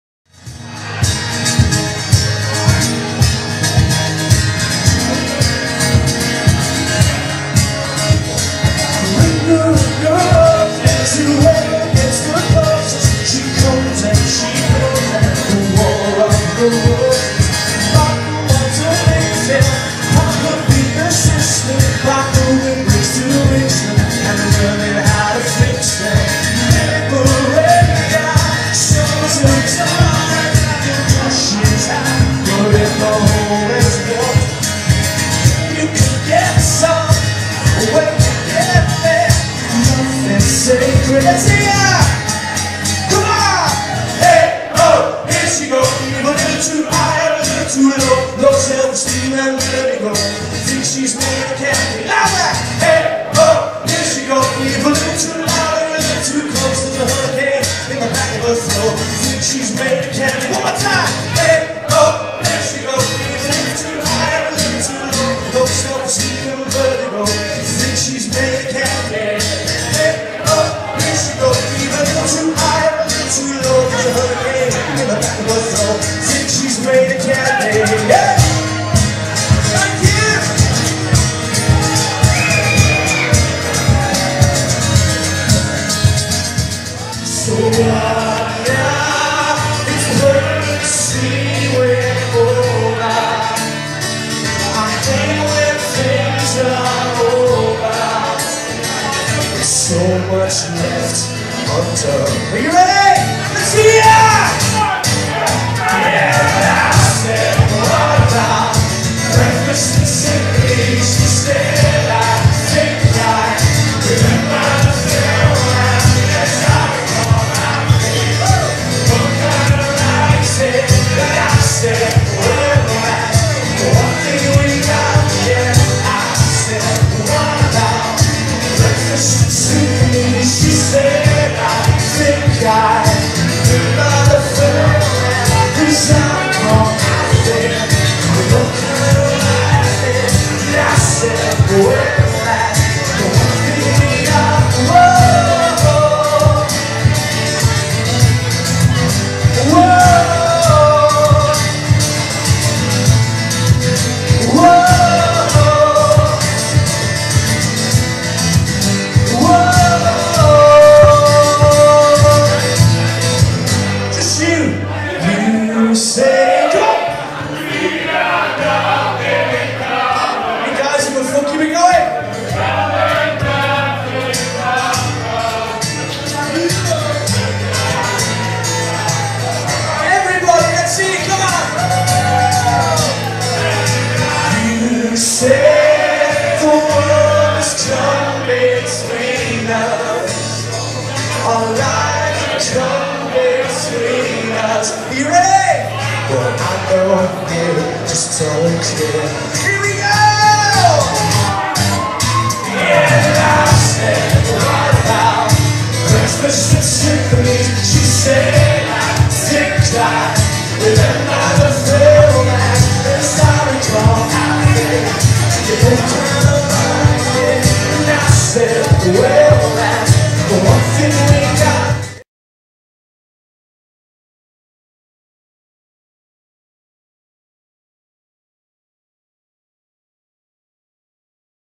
Barry Briercliffe - Live in Eric's, Liverpool
Barry Briercliffe performing live at Eric's on Mathew Street, Liverpool

Filmed on Saturday 8th December 2012